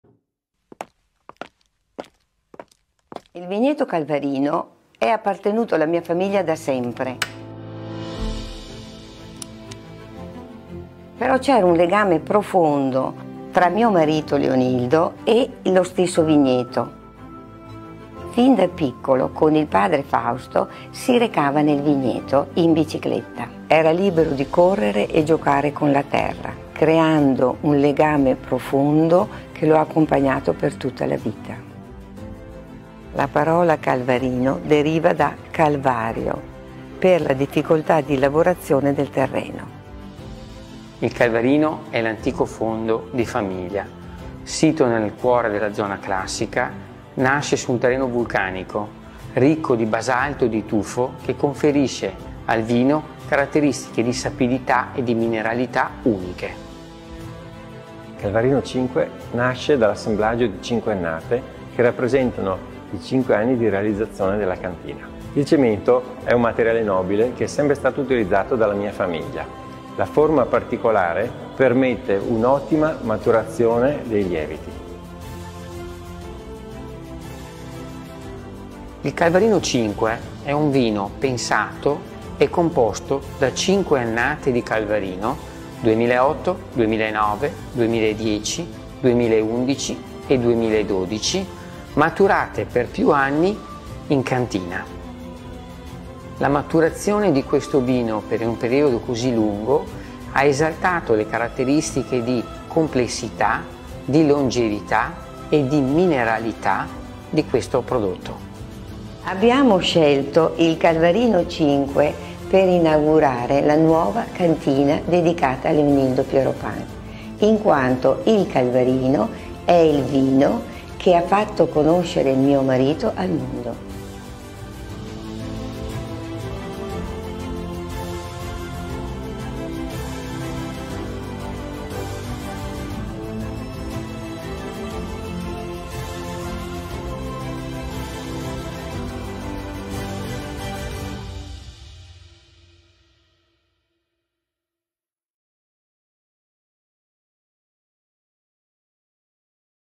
Pieropan C5 “Vini dell’Anima”
A remarkable wine 15 years in the making!

Calvarino 5 is the first wine of the “Vini dell’Anima” collection. Born from a project by Leonildo Pieropan and his sons Dario and Andrea, it is a blend of five Calvarino vintages (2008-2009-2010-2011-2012) aged for at least 10 years in large cement tanks on lees in the cellar.

Ageing this way in Pieropan’s cool cellars is slow and controlled.
This is a fascinating wine highlighting the incredible results blending across vintages can deliver.

Cool years add zip and freshness, warm years fuller flavours, complexity and textural elements. Time integrating all seamlessly.

The musical label connects the 5 vintages with a gentle flow. 50% of the blend comes from the cooler 2010 & 2008 vintages.

Tasting the 2011 as normally produced for the Calvarino vintage releases alongside the C5 made for a fascinating comparison. It has aged remarkably. On opening, it looked quite developed then quickly freshened in the glass to offer many of Calvarino’s hallmark traits. With the Pedigree of the Calvarino vineyard, it’s no surprise the C5 is so delicious!

I’ve been fortunate to see other white wines made following similar practices. Schloss Gobelsburg’s Tradition Heritage Cuvée 3 Years & the epic 50 Years iteration from Austria the most significant.

The production techniques yield wines with development and at the same time freshness.

The C5 looks like it is 3-5 years old not 15! The flowing, caressing, gentle texture and wonderful harmony, not typical for a young wine, give an inkling of its age. Rather than dissect it just enjoy the cashmere of wine.

A very delicate version of fine limoncello, herbs and spices add to a mille-feuille of flavours so seamlessly integrated that none stand out above another.

An incredibly complete, divine wine.